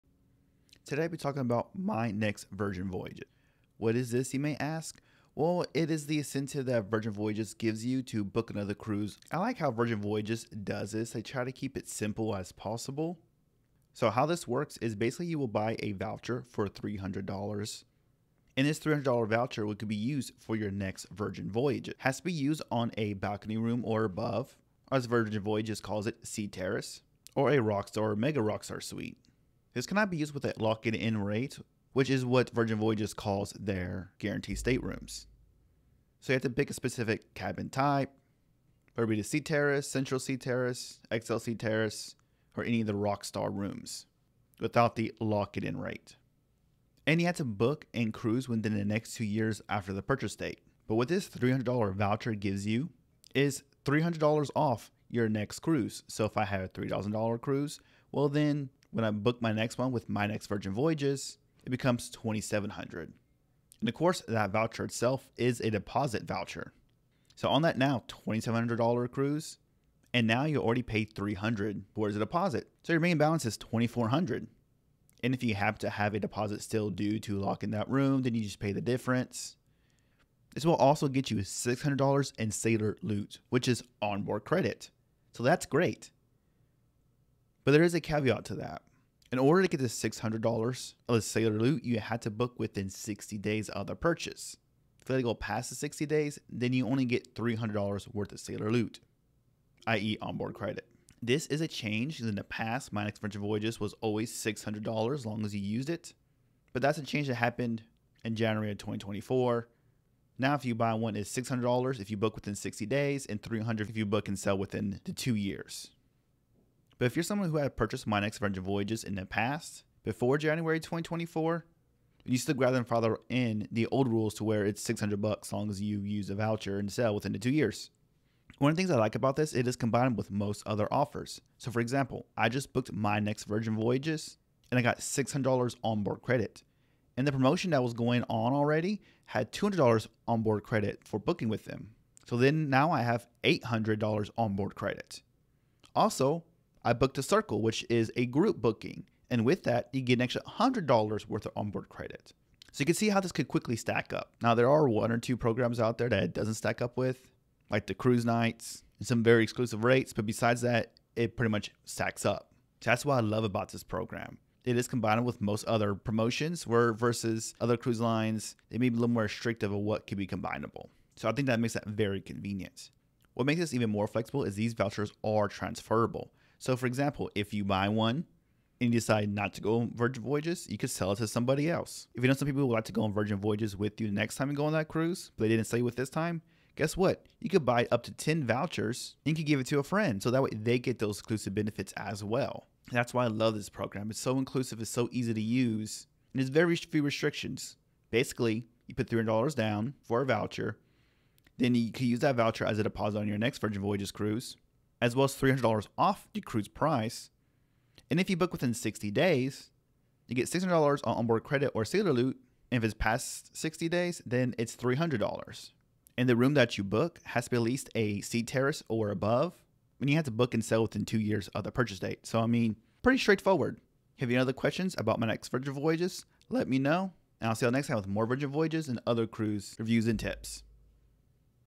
My Next Virgin Voyage Booking Bonus Explained
MNVV or My Next Virgin Voyage is their incentive program to encourage you to book your next cruise with Virgin Voyage. I will explain how this program works and why i love it!